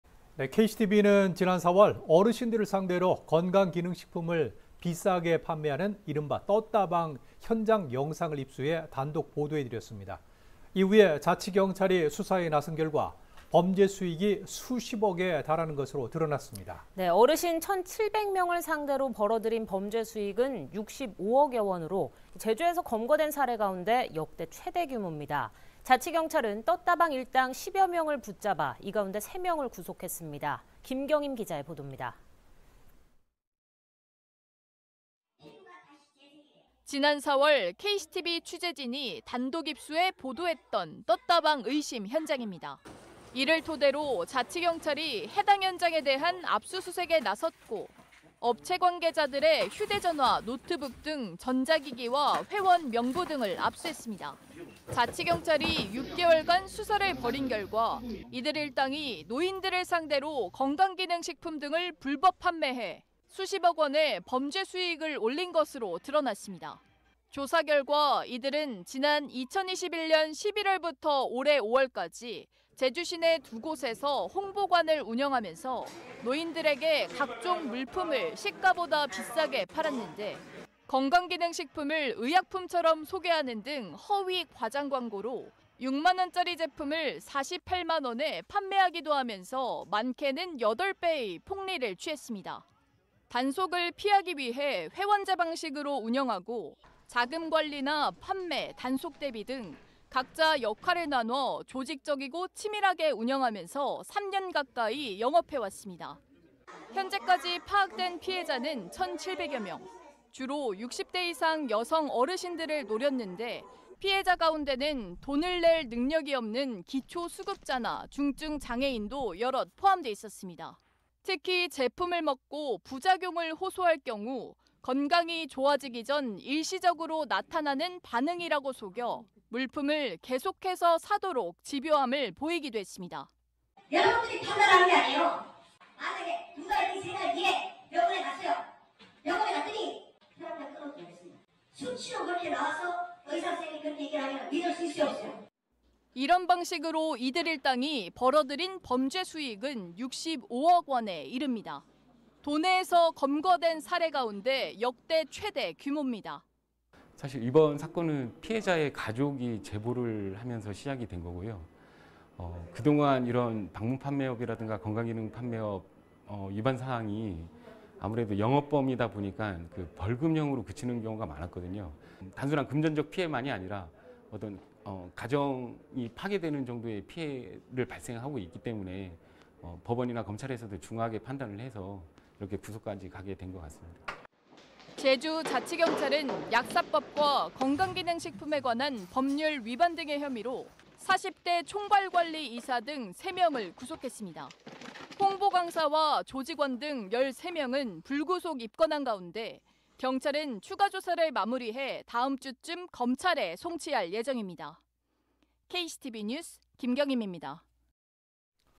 노인들 상대 65억 챙긴 '떴다방'…역대 최대 규모
KCTV는 지난 4월, 어르신을 상대로 건강기능 식품 등을 비싸게 판매하는 이른바 떴다방 의심 현장 영상을 입수해 단독보도해 드렸습니다.

이후 자치경찰이 수사에 나선 결과 이들이 어르신들을 상대로 수십억 원의 범죄 수익을 올린 것으로 드러났습니다.

어르신 1천 7백여 명을 상대로 벌어들인 범죄수익은 65여억 원으로 제주에서 검거된 사례 가운데 역대 최대 규모입니다.

자치경찰은 떴다방 일당 10여 명을 붙잡아 이 가운데 3명을 구속했습니다.

지난 4월, KCTV 취재진이 단독 입수해 보도했던 떴다방 의심 현장입니다.

이를 토대로 자치경찰이 해당 현장에 대한 압수수색에 나섰고,

업체 관계자들의 휴대전화, 노트북 등 전자기기와 회원 명부 등을 압수했습니다.

자치경찰이 6개월 간 수사를 벌인 결과, 이들 일당이 노인들을 상대로 건강기능식품 등을 불법 판매해 수십억 원의 범죄 수익을 올린 것으로 드러났습니다.

조사결과 이들은 지난 2021년 11월부터 올해 5월까지 제주시내 두 곳에서 홍보관을 운영하면서 노인들에게 각종 물품을 시가보다 비싸게 팔았는데,

건강기능식품을 의약품처럼 소개하는 등 허위 과장 광고로 6만 원 짜리 제품을 48만 원에 판매하기도 하면서 많게는 8배의 폭리를 취했습니다.

단속을 피하기 위해 회원제 방식으로 운영하고 자금관리나 판매, 단속 대비 등 각자 역할을 나눠 조직적이고 치밀하게 운영하면서 3년 가까이 영업해 왔습니다.

현재까지 파악된 피해자는 1천 7백여 명.

주로 60대 이상 여성 어르신들을 노렸는데, 피해자 가운데는 돈을 낼 능력이 없는 기초수급자나 중증장애인도 여럿 포함돼 있었습니다.

특히 제품을 먹고 부작용을 호소할 경우, 건강이 좋아지기 전 일시적으로 나타나는 반응이라고 속여 물품을 계속해서 사도록 집요함을 보이기도 했습니다.

[업체 관계자]
"여러분들이 판단하는 게 아니에요. 만약에 두 달 뒤 세 달 뒤에 병원에 갔어요. 병원에 갔더니 혈압약 끊어도 되겠습니다. 수치로 그렇게 나와서 의사 선생님이 그렇게 얘기를 하면 (제품 효과를) 믿을 수 있어요, 없어요?"

이런 방식으로 이들 일당이 벌어들인 범죄수익은 65여억 원에 이릅니다.

도내에서 검거된 사례 가운데 역대 최대 규모입니다.

[박태언 / 제주자치경찰단 기획민생수사팀장]
"사실 이번 사건은 피해자의 가족이 (언론에) 제보를 하면서 시작이 된 거고요. 그동안 이런 방문판매업이라든가 건강 기능(식품) 판매업 위반 사항이 아무래도 영업범이다 보니까 벌금형으로 그치는 경우가 많았거든요. 단순한 금전적 피해만이 아니라 가정이 파괴되는 정도의 피해가 발생하고 있기 때문에 법원이나 검찰에서도 중하게 판단해서 구속까지 가게 된 것 같습니다."

제주자치경찰은 약사법과 건강기능식품에 관한 법률 위반 등의 혐의로 40대 총괄 관리 이사 등 3명을 구속했습니다.

홍보강사와 조직원 등 13명은 불구속 입건한 가운데 경찰은 추가 조사를 마무리해 다음주 쯤 검찰에 송치할 예정입니다.